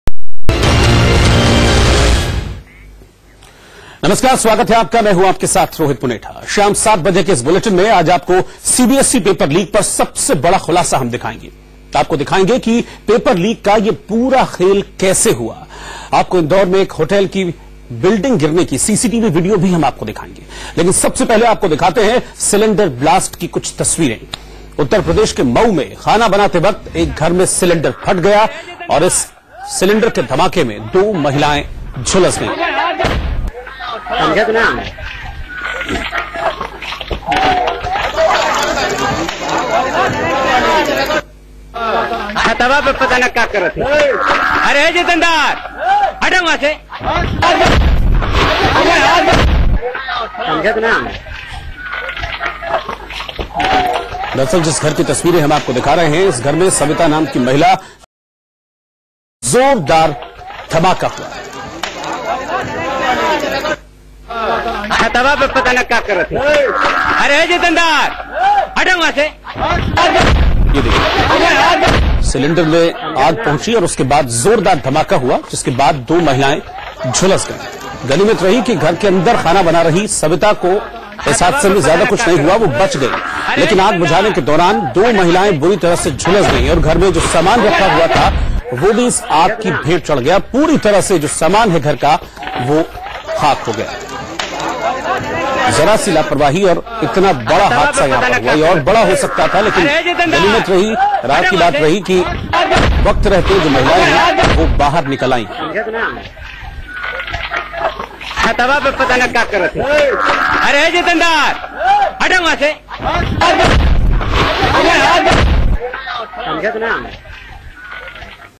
यूपी के मऊ में खाना बनाते वक्त एक घर में फटा सिलेंडर; झुलस गईं महिलाएं
आपको दिखाते हैं सिलेंडर ब्लास्ट की तस्वीरें। यूपी के मऊ में खाना बनाते वक्त एक घर में सिलेंडर फट गया...जिसमें दो महिलाएं झुलस गईं.। सविता नाम की महिला चूल्हे पर खाना पका रही थी...। इसी बीच उसे बच्चे की रोने की आवाज़ आई...वो बच्चे को चुप कराने के लिए गई। उसी वक्त चूल्हे से निकली आग सिलेंडर तक पहुंची...जिससे सिलेंडर फट गया...और जोरदार धमाका हुआ । गनीमत रही कि घर के अंदर खाना बना रही सविता को इस हादसे में बच गई...लेकिन आग बुझाने के दौरान दो महिलाएं बुरी तरह झुलस गईं...।